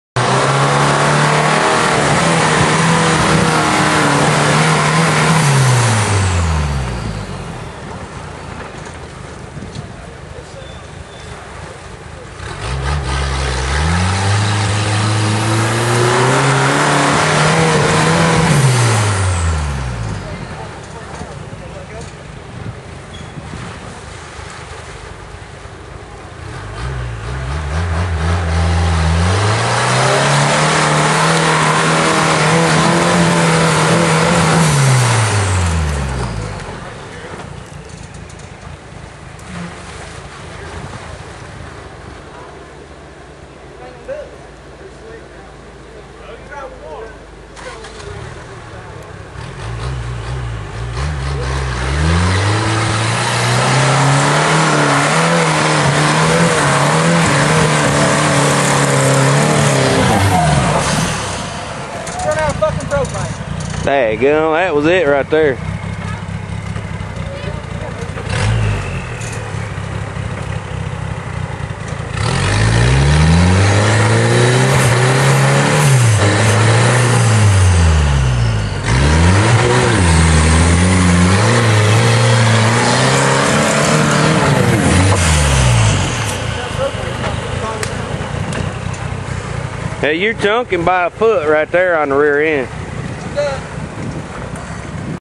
Jerry Prevatt on the new creek hill at Lewis Chapel
A 4 banger hill climber show nuff